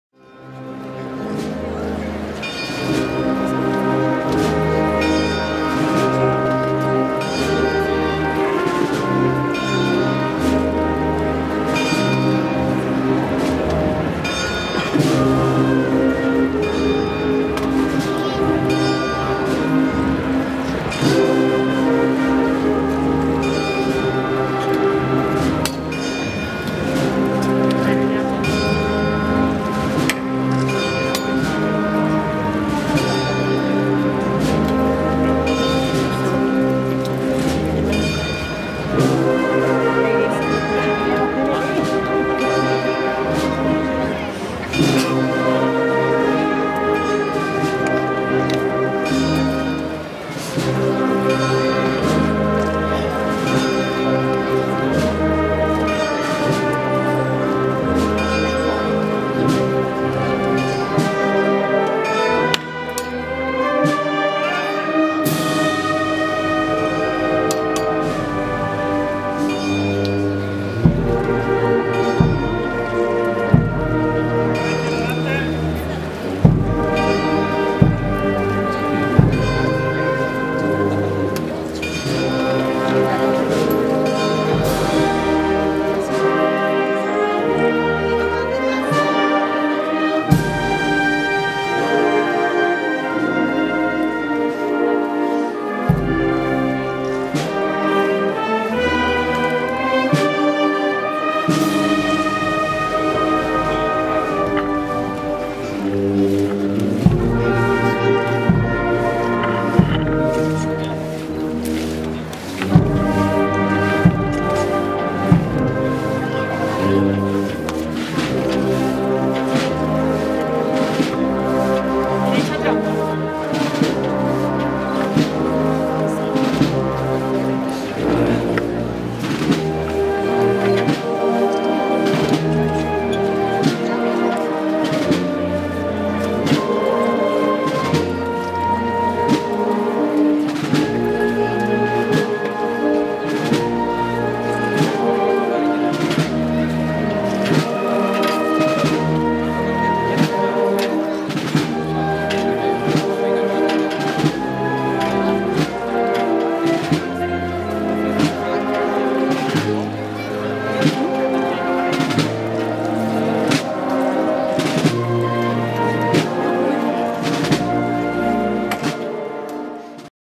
Salida Santo Entierro de Huelva en la Magna
Salida del Paso del Santo Entierro de Huelva, de camino a la Procesión Magna de Huelva con motivo del año de la Misericordia, en la tarde del sábado 17 de Septiembre de 2016.